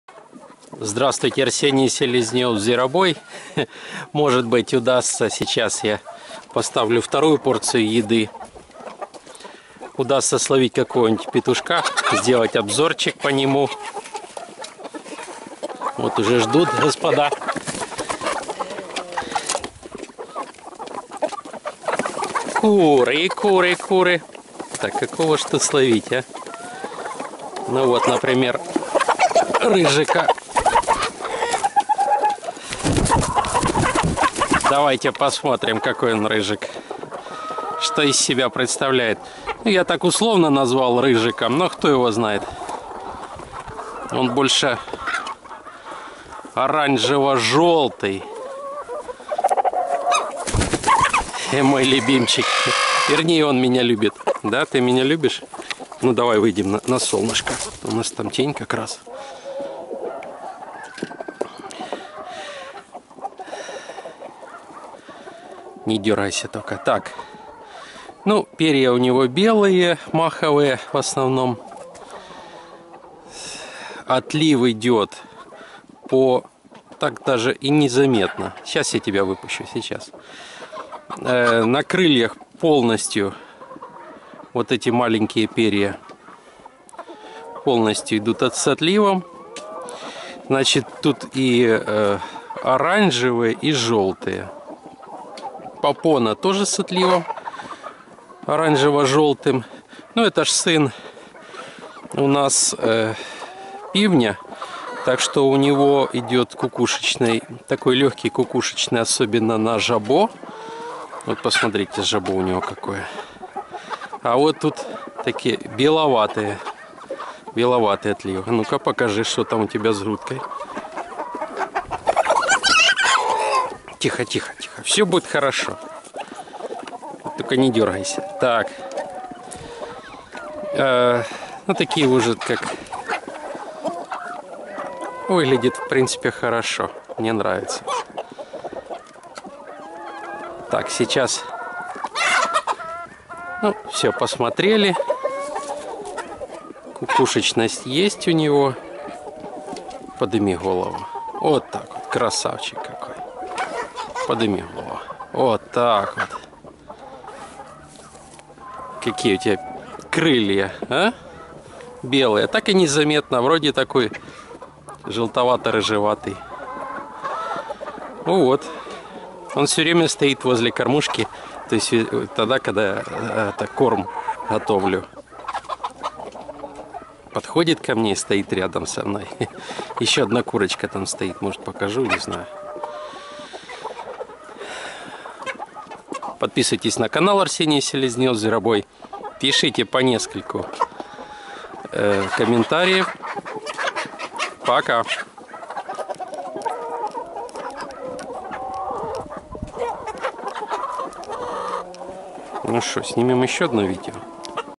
Обзор: "Я не Рыжиков, а Чижиков!" | Интересный петушок
Кукушечный, со светло жёлтым и оранжевым отливом. Ярко рыжие и белые перья. Размер средний. Сын Півня.